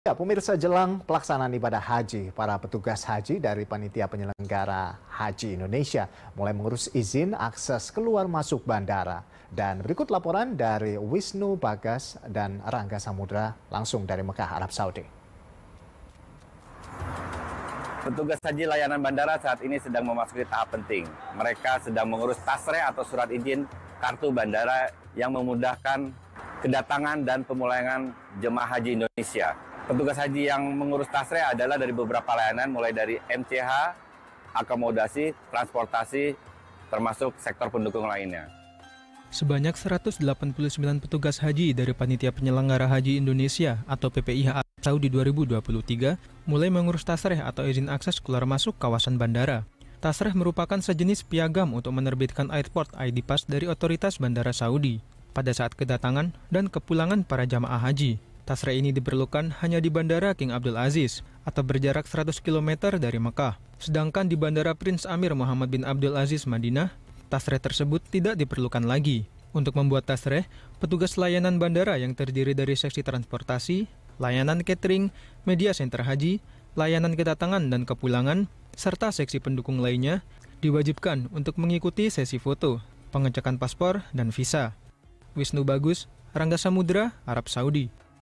189 Petugas Haji Indonesia Urus Izin Akses Keluar Masuk Kawasan Bandara
Jelang pelaksanaan ibadah haji, para petugas haji dari Panitia Penyelenggara Haji Indonesia (PPIH) mulai mengurus izin akses keluar masuk bandara. Sebanyak 189 petugas haji Indonesia mengurus izin akses yang hanya berlaku di Bandara King Abdulaziz, Mekkah.